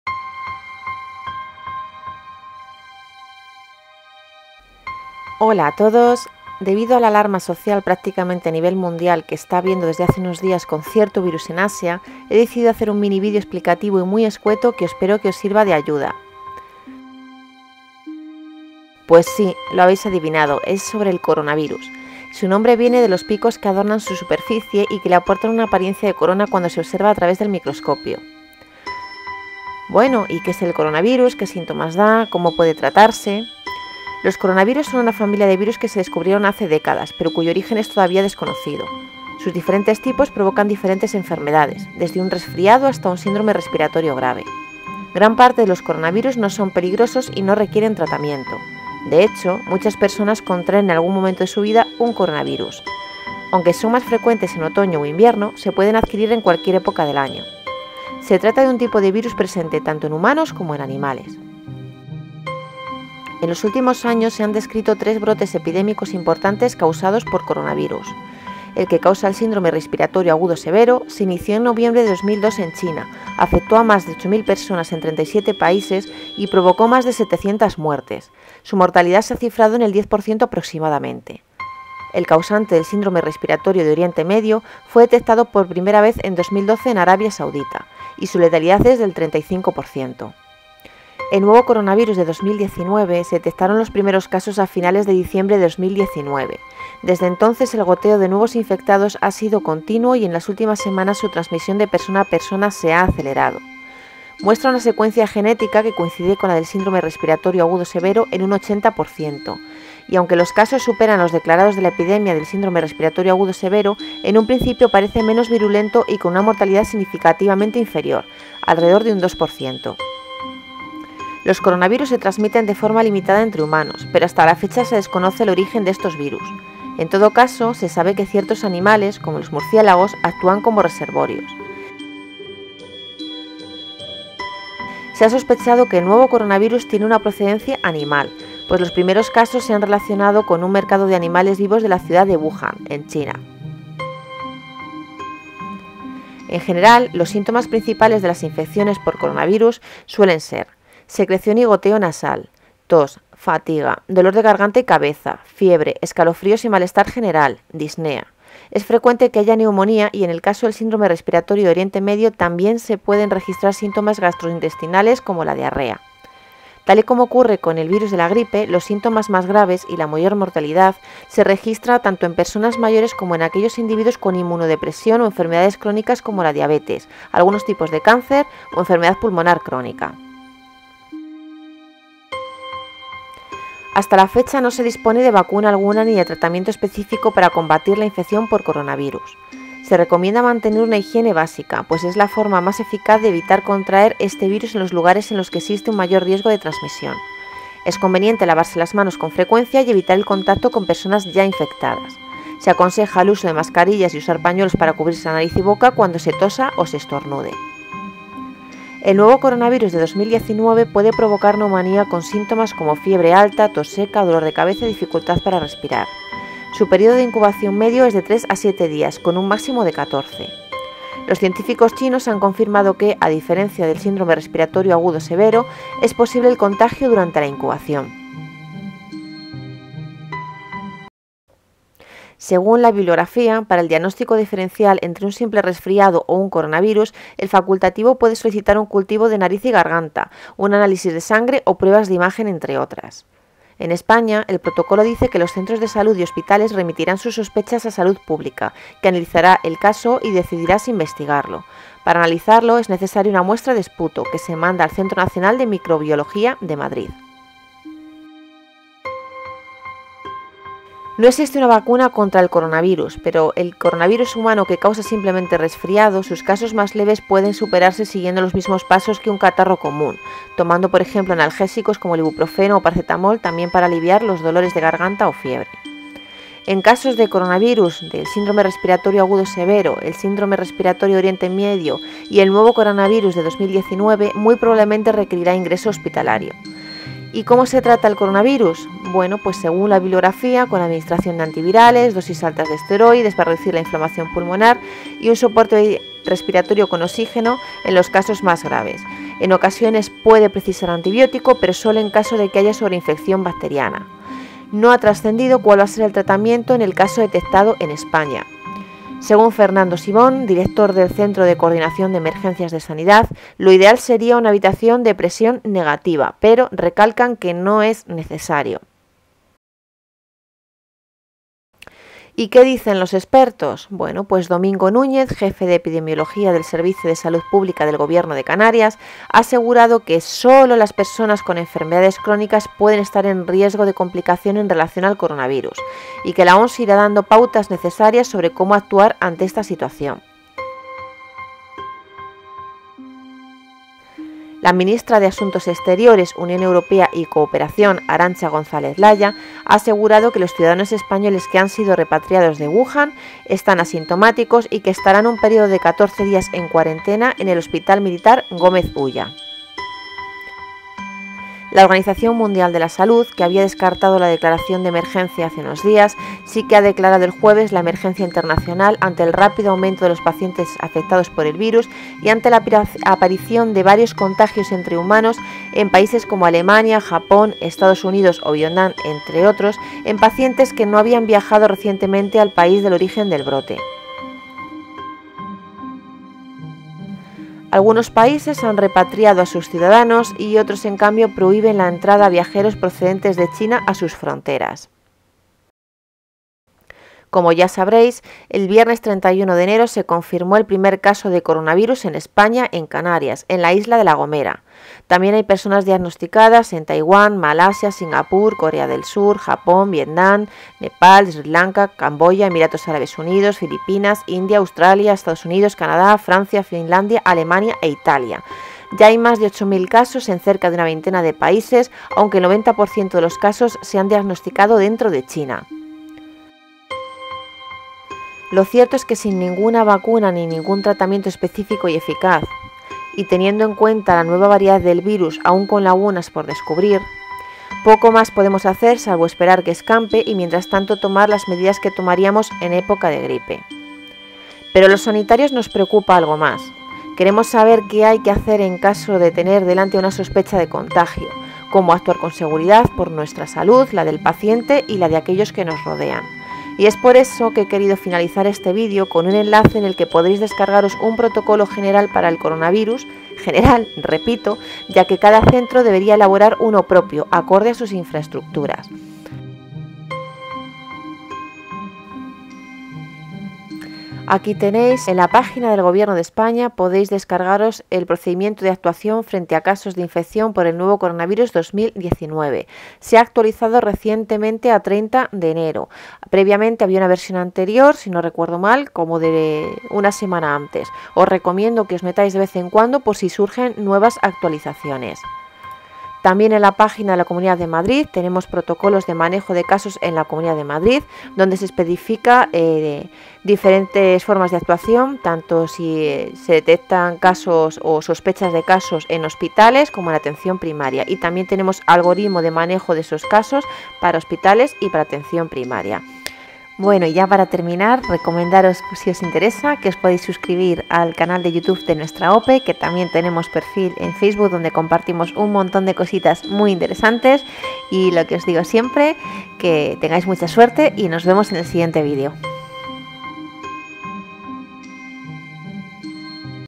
CORONAVIRUS - Información básica y útil
En este video os hablo de las características del Coronavirus, los protocolos de actuación en España y los datos elementales de este virus.